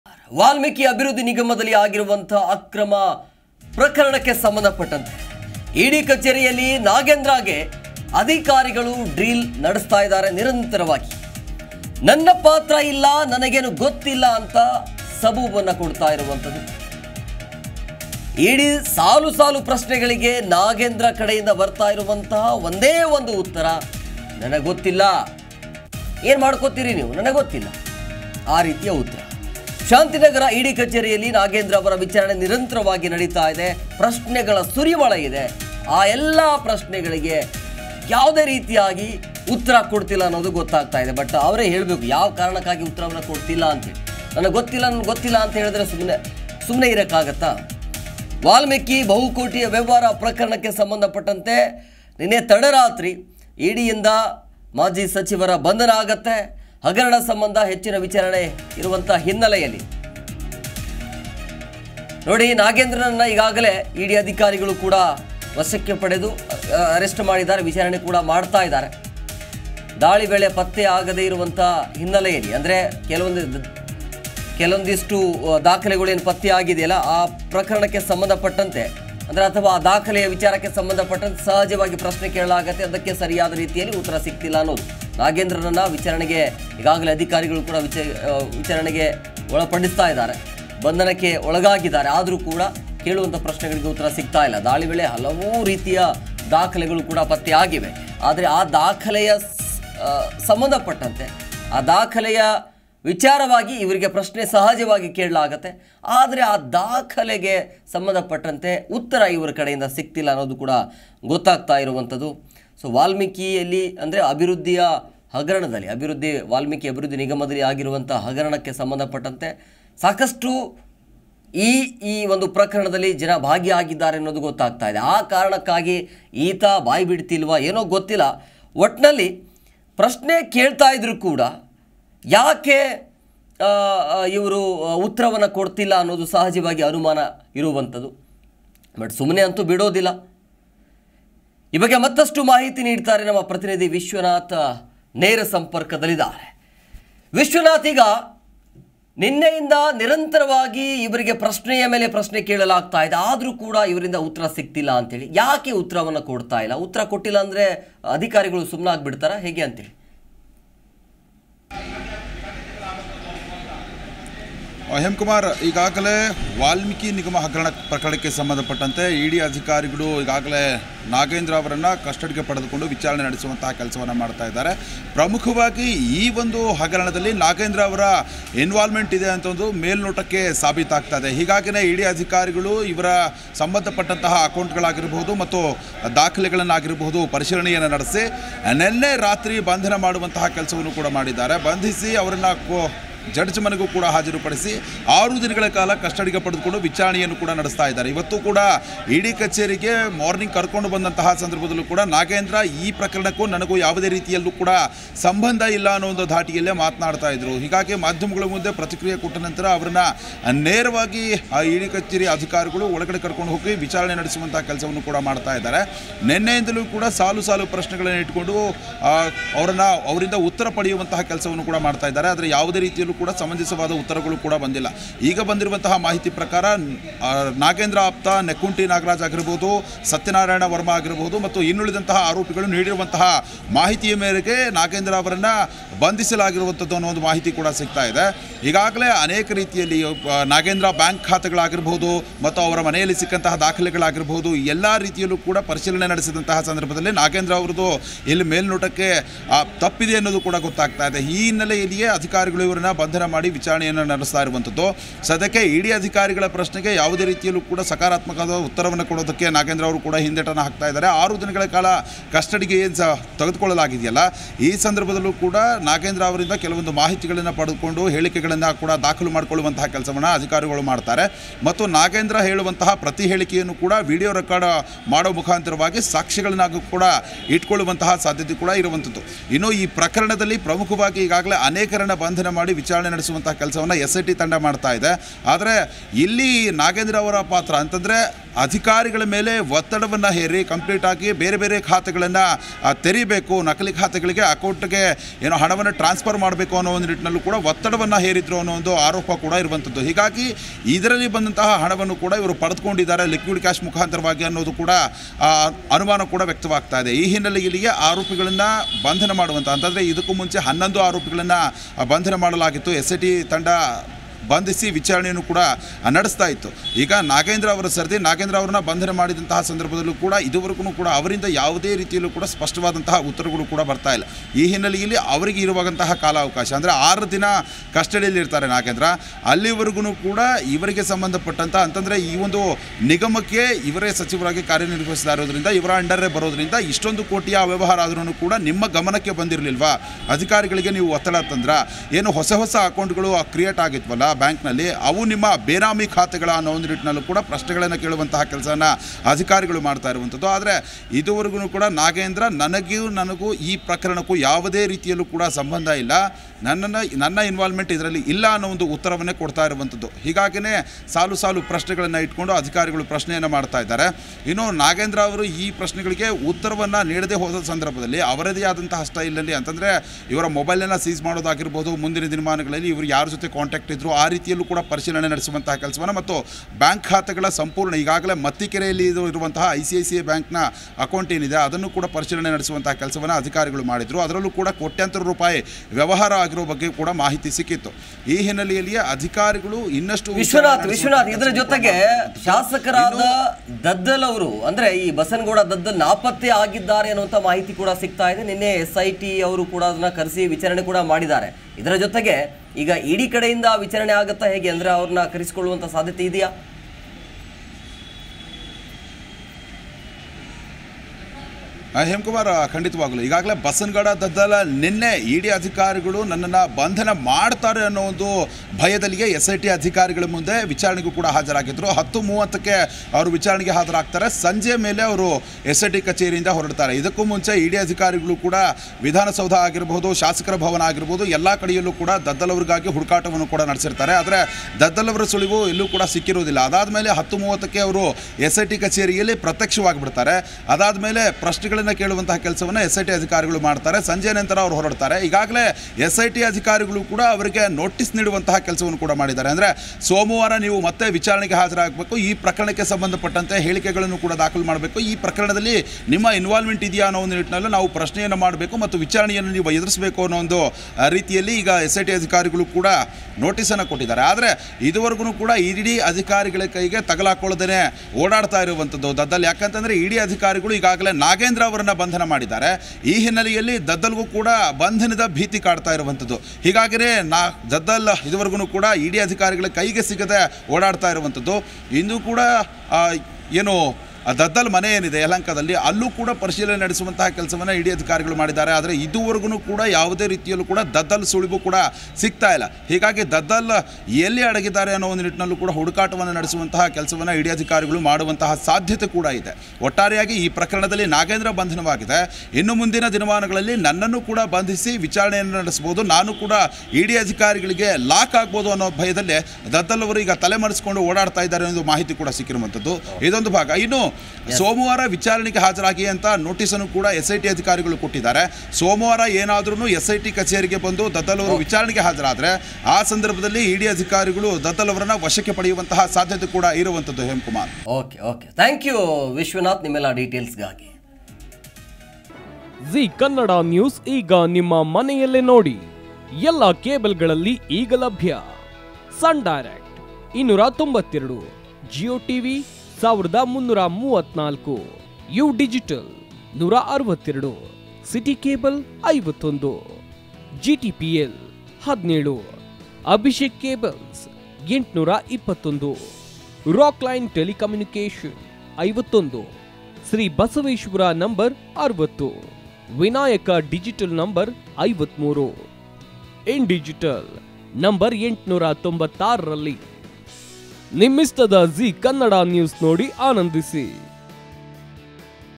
Nagendra | ED Custody | ನನ್ನ ಪಾತ್ರವಿಲ್ಲ.. ನನಗೆ ಗೊತ್ತಿಲ್ಲ ಎಂದು ಸಬೂಬು
ಇಡಿ ಕಚೇರಿಯಲ್ಲಿ ನಾಗೇಂದ್ರಗೆ ಅಧಿಕಾರಿಗಳು ಡ್ರಿಲ್‌